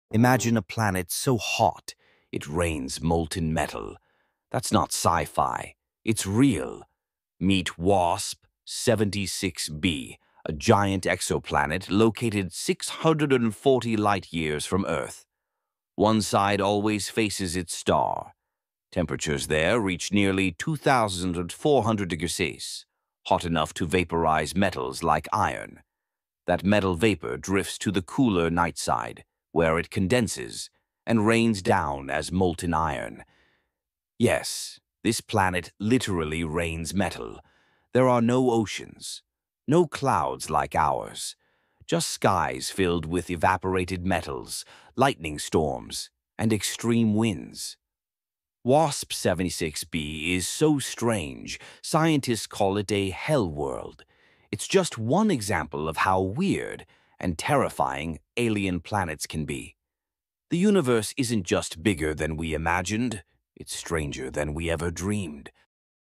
It Rains Molten Iron on This Planet
Imagine a planet where it literally rains molten metal.
Welcome to WASP-76b — a giant exoplanet 640 light-years away.

One side always faces its star, reaching temperatures over 2,400°C.
That heat vaporizes metals like iron, which then condense on the night side…
And fall as molten iron rain.

Scientists call it a hell world.

There are no oceans. No water.
Just glowing metal storms, scorching winds, and alien skies.

This is not science fiction — this is our universe.

💬 What would you name this metal rain planet?